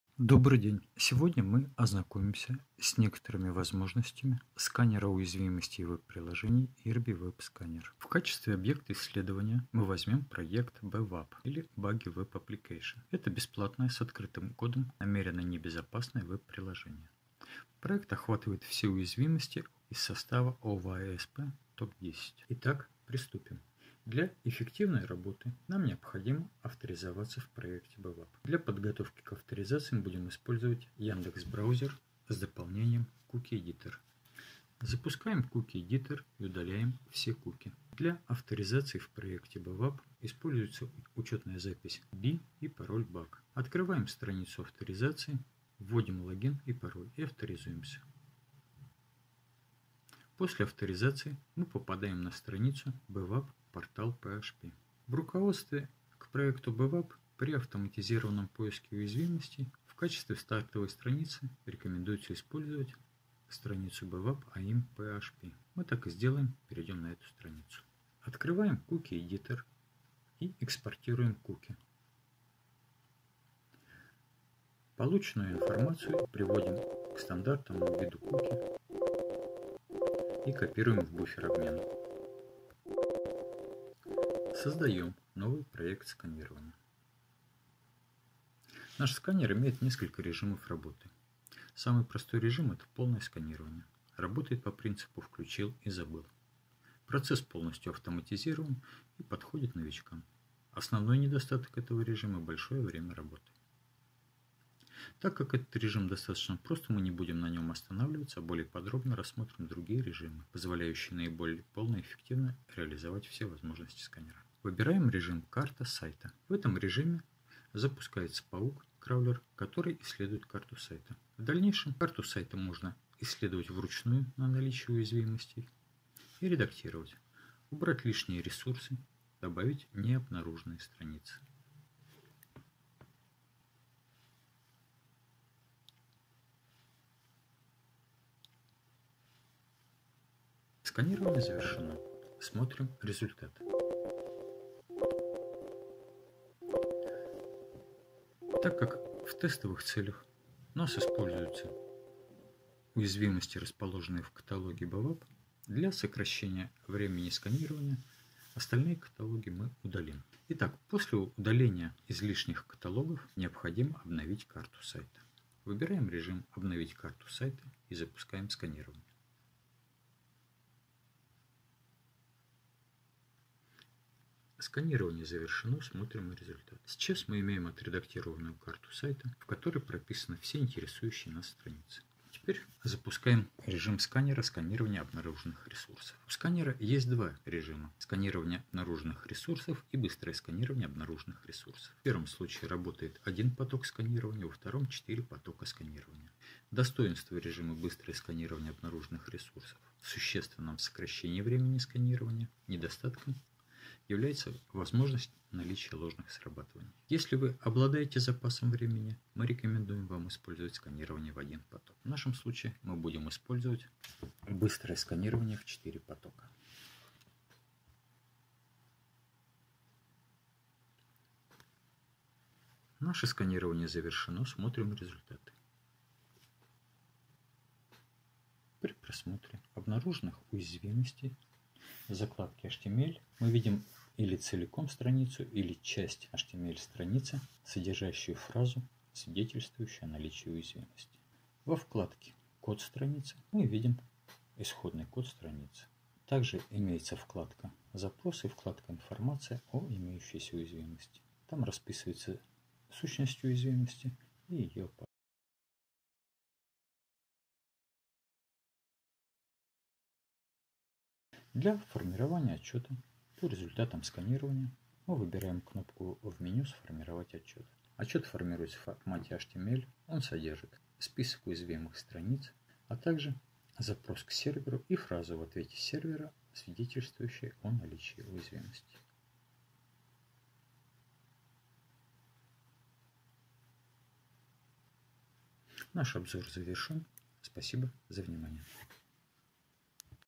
Пентест web приложений. Использование веб сканера. Поиск уязвимостей веб сайтов (XSS, SQL Injection)
Автоматизированный поиск уязвимостей сайтов (веб-приложений). Практический урок использования веб сканера IWS (Irbby Web Vulnerability Scanner),